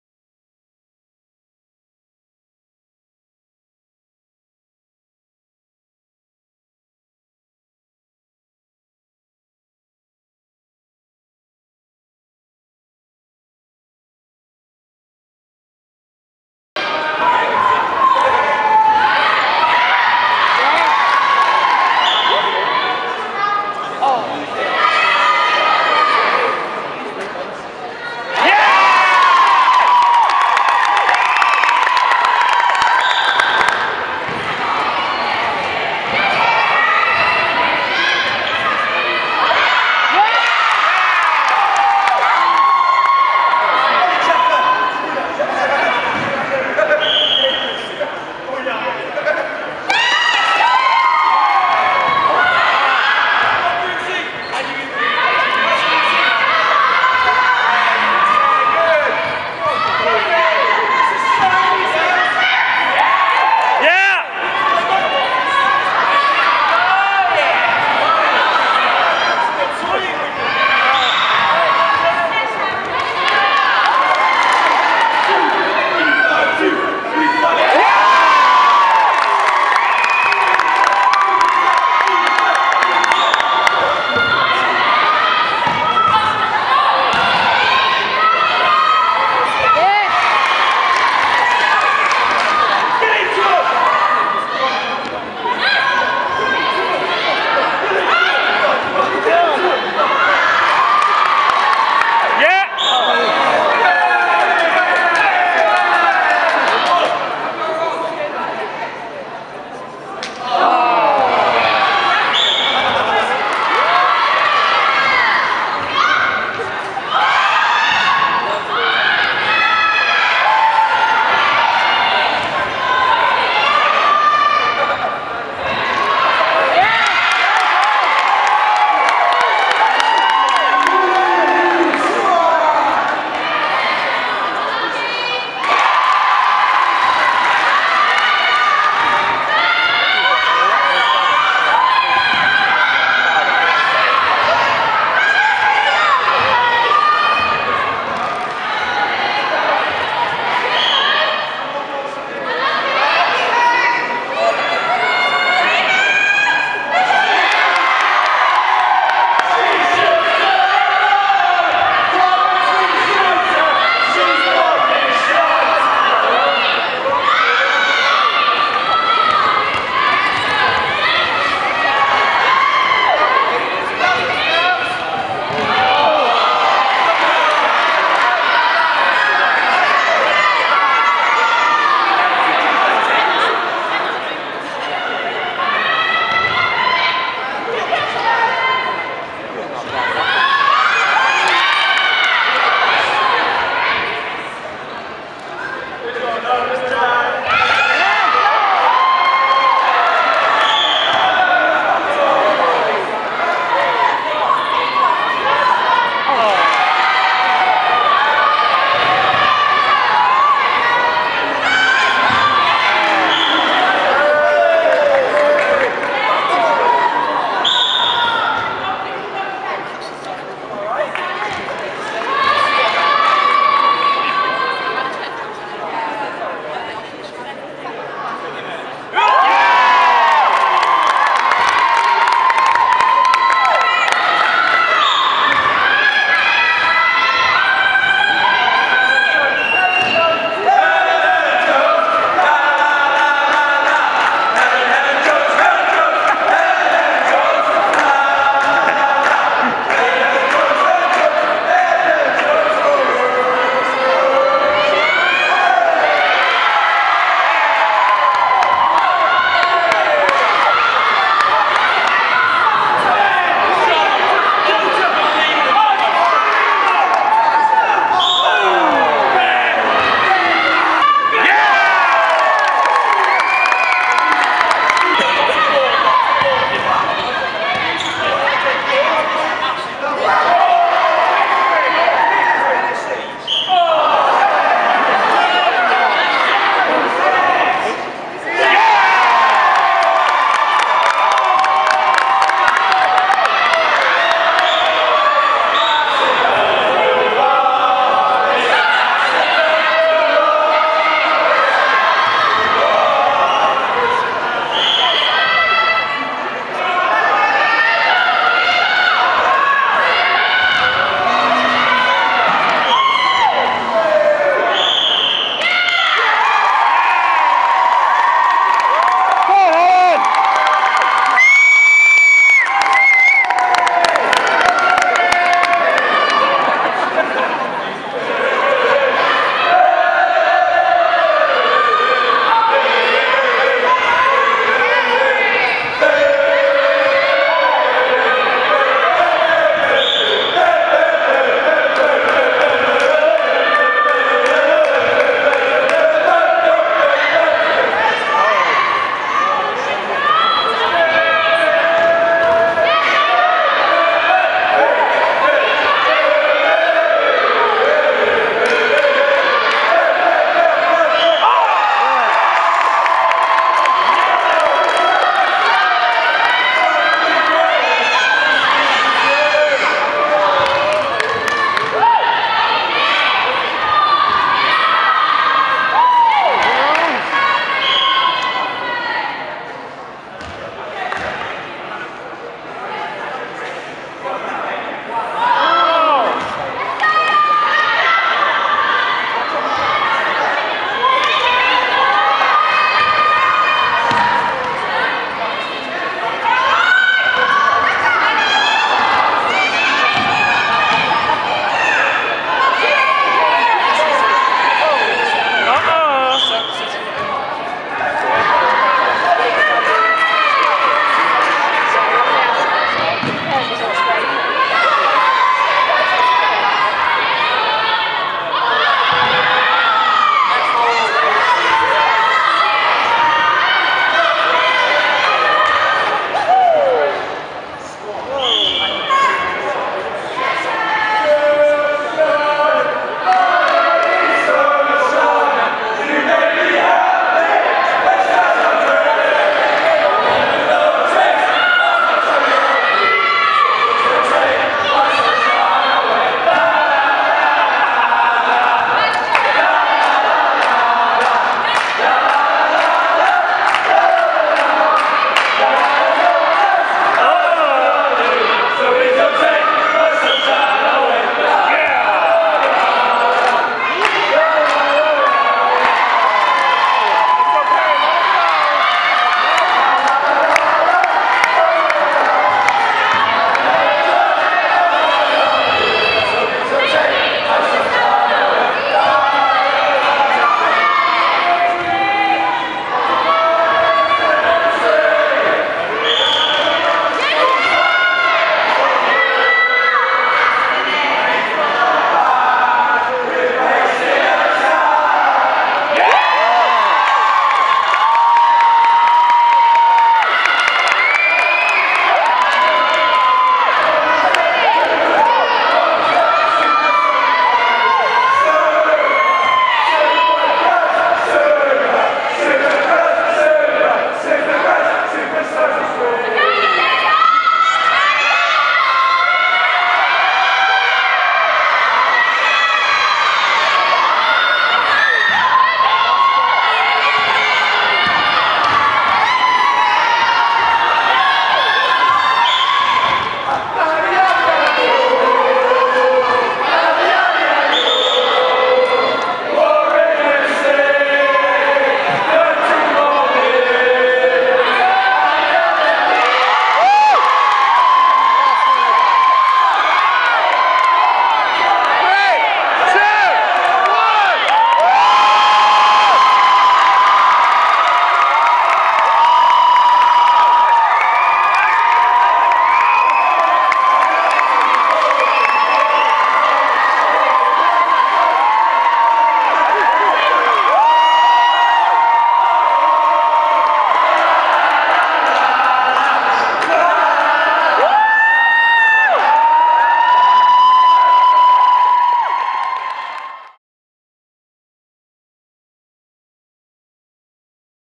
Warwick vs Coventry Varsity Netball 2017
A selection of clips from the Warwick vs Coventry Varsity Netball game 2017.